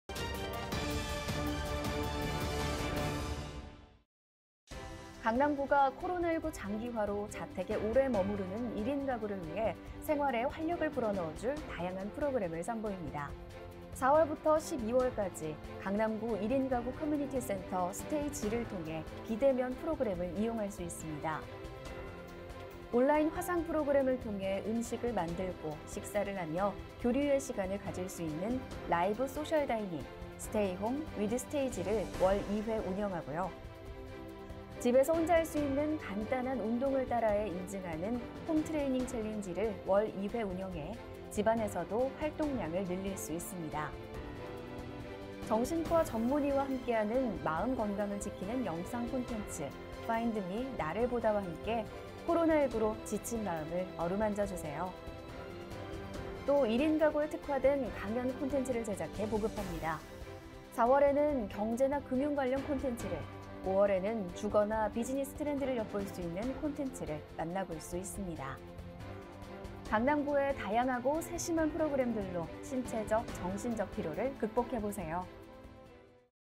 코로나19 극복 1인가구 활력 프로그램 운영
강남구가 코로나19 장기화로 자택에 오래 머무르는 1인 가구를 위해 생활에 활력을
불어넣어줄 다양한 프로그램을 선보입니다.

4월부터 12월까지 강남구 1인 가구 커뮤니티센터 '스테이.지'(STAY.G)를 통해 비대면 프로그램을
이용할 수 있습니다.

온라인 화상 프로그램을 통해 음식을 만들고 식사를 하며 교류의 시간을 가질 수 있는 라이브
소셜다이닝 ‘Stay Home, With STAY.G’를 월 2회 운영하고요.

집에서 혼자할 수 있는 간단한 운동을 따라해 인증하는 ‘홈트레이닝 챌린지’를 월 2회 운영해
집안에서도 활동량을 늘릴 수 있습니다.

정신과 전문의와 함께하는 마음건강을 지키는 영상 콘텐츠 ‘Find me,
나를 보다’와 함께 코로나19로 지친 마음을 어루만져주세요.

또 1인가구에 특화된 강연 콘텐츠를 제작해 보급합니다. 4월에는 경제나
 금융 관련 콘텐츠를, 5월에는 주거나 비즈니스 트렌드를 엿볼 수 있는
콘텐츠를 만나볼 수 있습니다.

강남구의 다양하고 세심한 프로그램들로 신체적·정신적 피로를
극복해보세요!